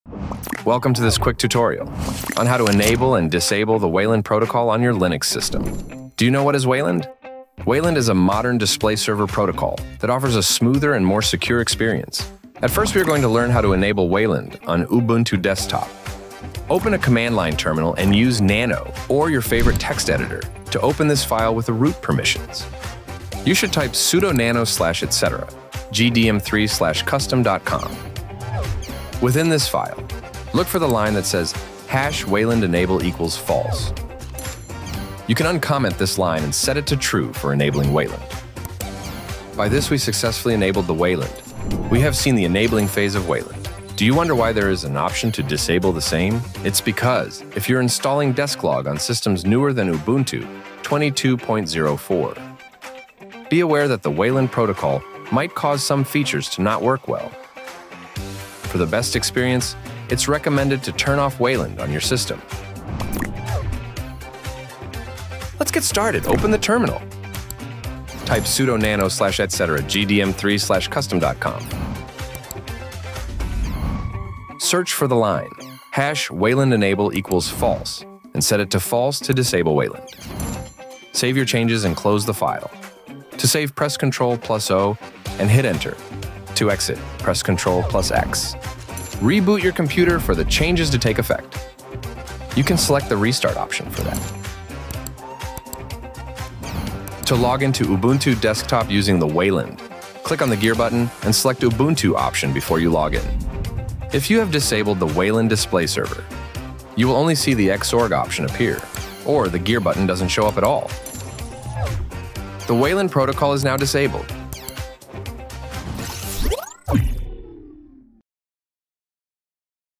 How to disable Wayland Protocol On Ubuntu ? Complete Guide
Welcome to your ultimate guide on how to disable the Wayland protocol in Ubuntu! Whether you're new to Ubuntu or a long-time user, this video will walk you through the straightforward steps to switch from Wayland to the Xorg display server.

We’ll also show you how to ensure the process is smooth and error-free, helping you optimize your system’s performance and compatibility with various applications.